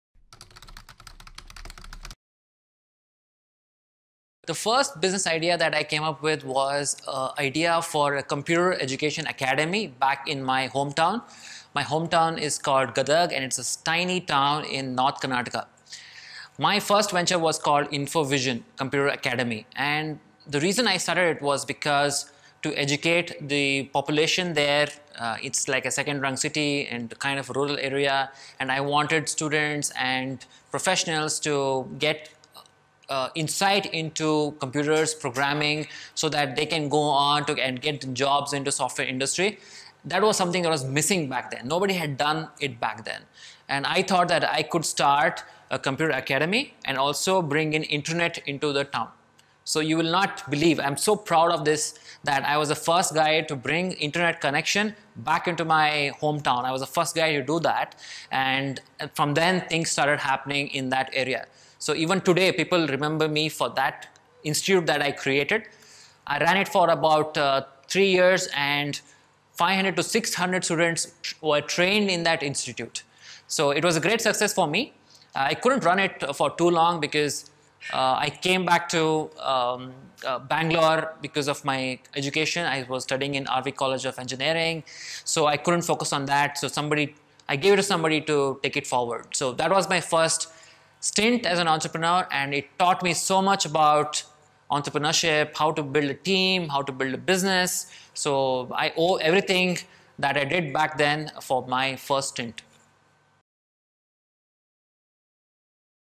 My First Business Idea
Your first business idea is like your first crush. You can't really seem to get over it. So, to see my first business idea come to fruition was something magical. Check out the video and comment if you've had a similar experience.